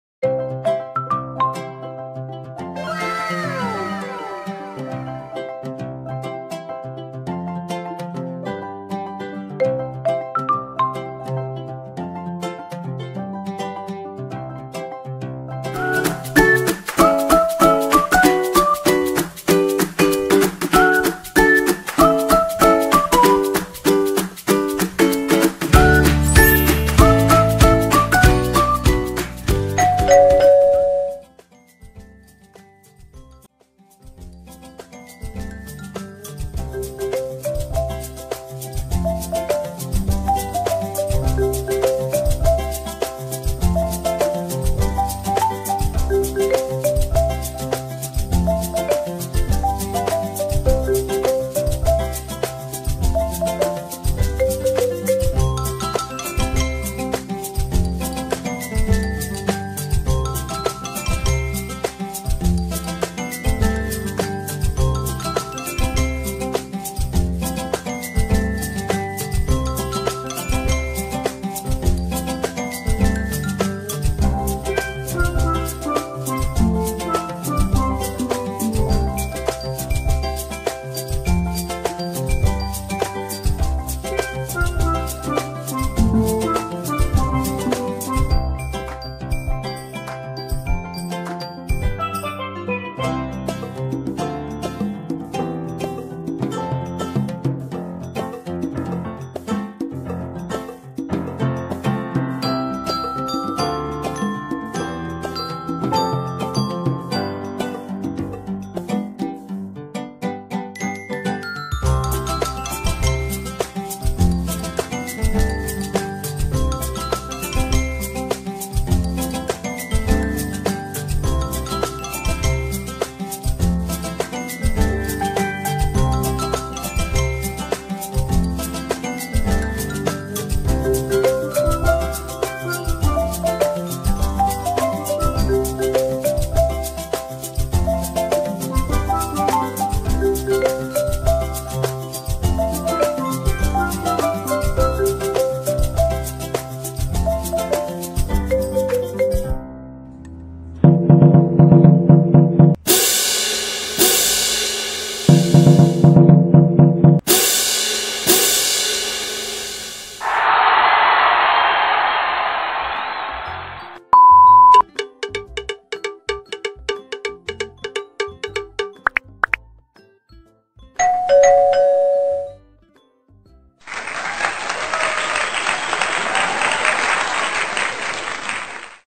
5粒鸡蛋的故事｜The Egg's Story
鸡蛋焖一焖，环绕着浓浓的腐竹与马铃薯的甜味😋好下饭😍

食材：
腐竹
蒜末
姜片
洋葱
鸡蛋
马铃薯

调味：
酱油 x 2餐匙
耗油 x 2餐匙
黑醋 x 2餐匙
糖 x 1餐匙
盐 （少许）

**以上调味都是根据个人喜爱和口味而调哦🤗